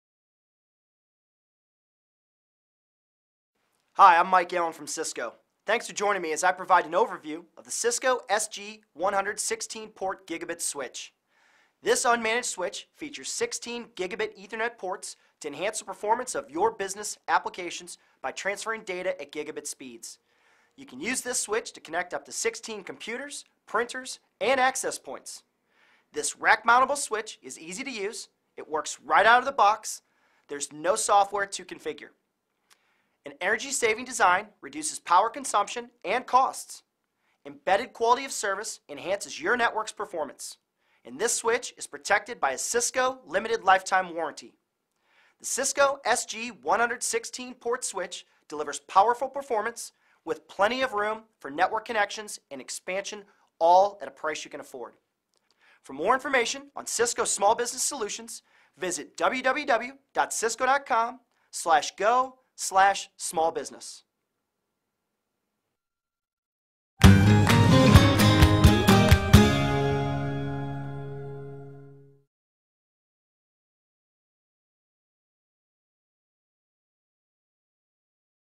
Cisco Small Business SG 100-16 Switch
This Cisco SG 100-16 switch is easy to set up. It works right out of the box; theres no software to configure.